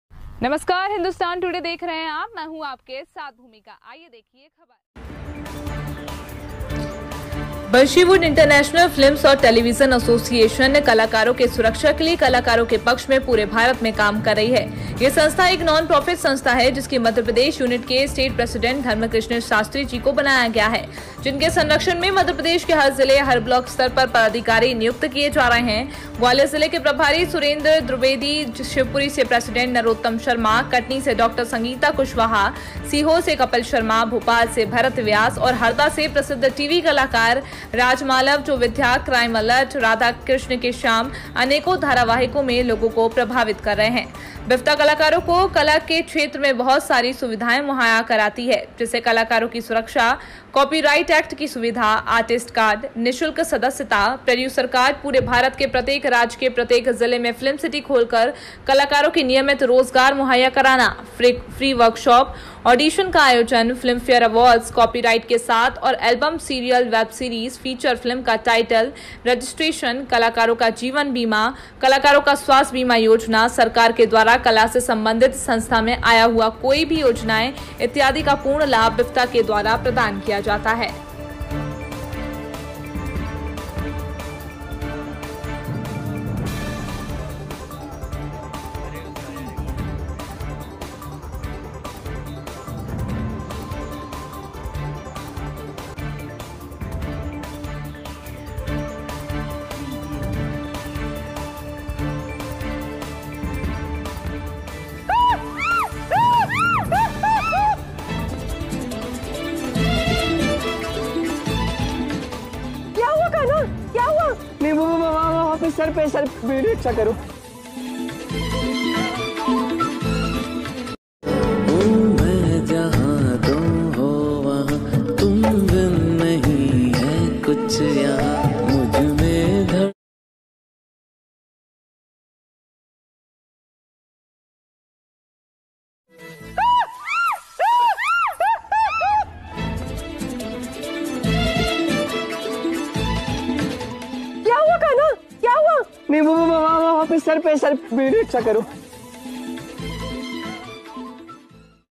म.प्र.के हर जिले में BIFTAखोलेगी प्रोडक्शन हाउसग्रामीण प्रतिभाओं को मुफ्त एक्टिंगसीखने व काम का मौका
:म.प्र.के हर जिले में BIFTA खोलेगी प्रोडक्शन हाउस
ग्रामीण प्रतिभाओं को मुफ्त एक्टिंग, सीखने व काम का मौका

बंशीवुड इंटरनेशनल फिल्म्स एंड टेलीविजन एसोसिएशन (BIFTA) कलाकारो के सुरक्षा के लिए कलाकारो के पक्ष में पूरे भारत में काम कर रही है । यह संस्था एक नॉन प्रॉफिट संस्था है।जिसके मध्यप्रदेश यूनिट के स्टेट प्रेसिडेंट धर्मकृष्ण शास्त्री जी को बनाया गया है। जिनके संरक्षण में मध्यप्रदेश के हर जिले हर ब्लॉक स्तर पर पदाधिकारी न्युक्त किए जा रहे हैं।ग्वालियर जिले के प्रभारी सुरेन्द्र दूबेदी जी, शिवपुरी से प्रेसिडेंट नरोत्तम शर्मा,कटनी से डॉ.संगीता कुशवाह,सीहोर से कपिल शर्मा जी भोपाल से भारत व्यास  जी और हरदा से प्रसिद्ध टीवी कलाकार राज मालव जो विद्या ,क्राइम अलर्ट ,राधा के श्याम ,अनेकों धारावाहिकों में लोगो को प्रभावित कर रहे हैं।BIFTA कलाकारो को कला के क्षेत्र में बहुत सारी सुविधाएं मुहैया कराती है जैसे -:कलाकारो की सुरक्षा,कॉपीराइट एक्ट की सुविधा,आर्टिस्ट कार्ड(कलाकार कार्ड),निशुल्क सदस्यता, प्रोड्यूसर कार्ड,पूरे भारत के प्रत्येक राज्य के प्रत्येक जिले में फिल्म सिटी खोलकर कलाकारो की नियमित रोजगार मुहैया कराना, फ्री वर्कशॉप (एक्टिंग,सिंगिंग,डांसिंग),ऑडिशन का आयोजन,फिल्मफेयर अवार्ड्स,कॉपीराइट के साथ एड एलबम सीरियल वेब सीरीज फीचर फिल्म का टाइटल रजिस्ट्रेशन,कलाकारो का जीवन बीमा योजना,कलाकारो का स्वास्थ्य बीमा योजना,सरकार के द्वारा कला से संबधित संस्था में आया हुआ कोई भी  योजनाओं  इत्यादि  का पूर्ण लाभ BIFTA के द्वारा प्रदान किया जाता है।
[7/21, 8:42 PM] Kamal Sahu Bhitarbar: मध्यप्रदेश के जिला एवं ब्लॉक स्तर पर अभिनय के क्षेत्र को विकसित करेगे मध्यप्रदेश BIFTA प्रेसिडेंट धर्मकृष्ण शास्त्री जी